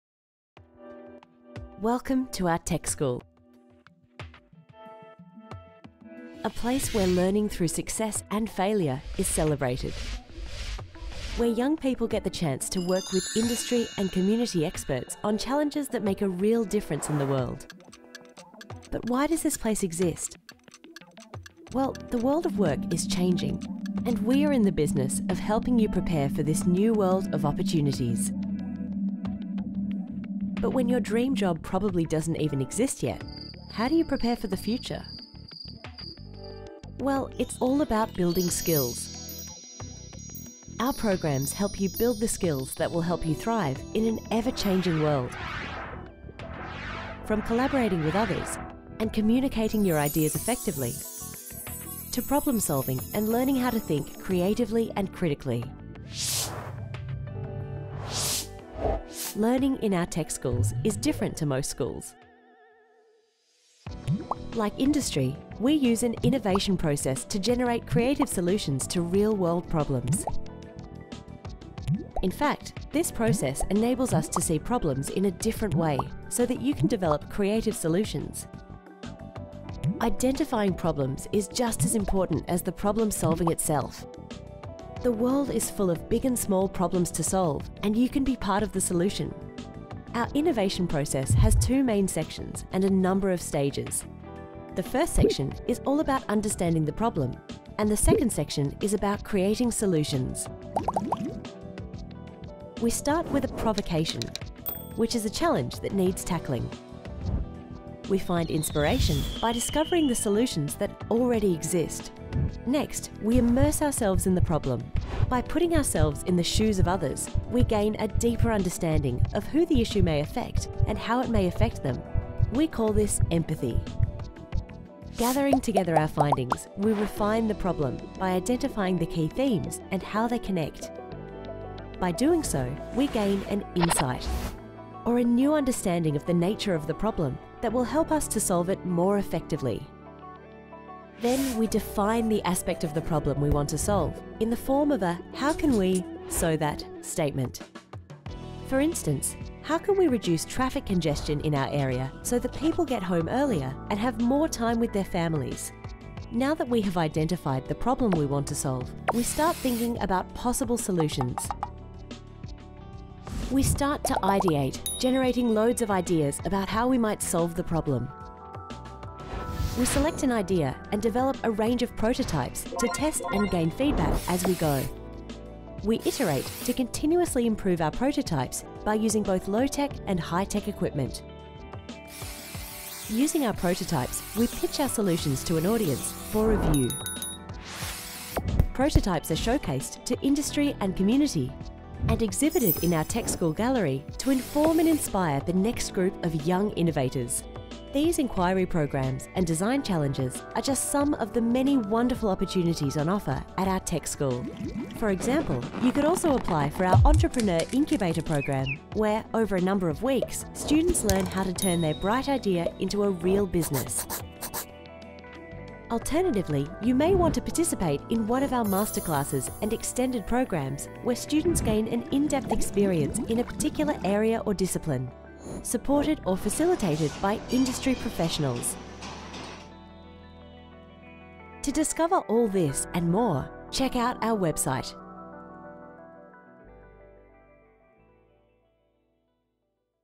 Whittlesea Tech School Innovation Process
Animation illustrating how Whittlesea Tech School is preparing young people for the future world of work through the Melbourne Polytechnic Tech Schools Innovation Process.